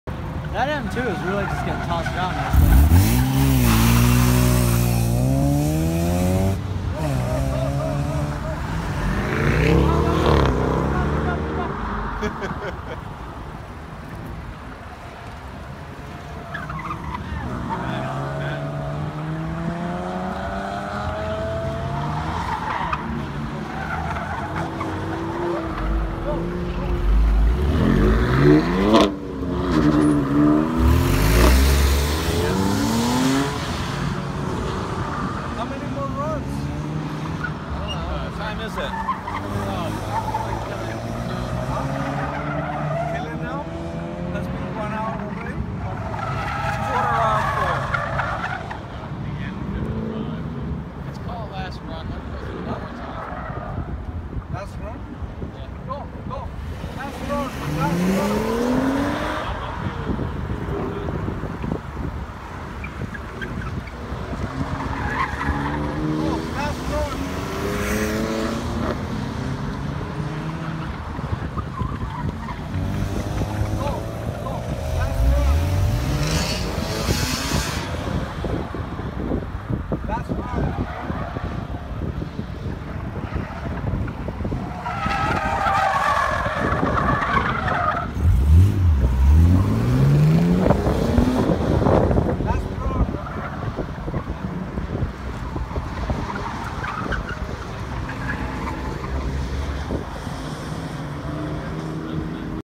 Boston BMW CCA - ADSS - 1985 Mazda Rx7
This is from the final activity of the BMW CCA Advanced Driver Skills School where we combined all the training into a lap around an auto cross course.

Lap times:
Me:  34.82 / 33.27
White BMW 1: 39.70 / 38.83
White BMW 2: 35.04 / 33.91
Black Porsche: 34.15 / 33.46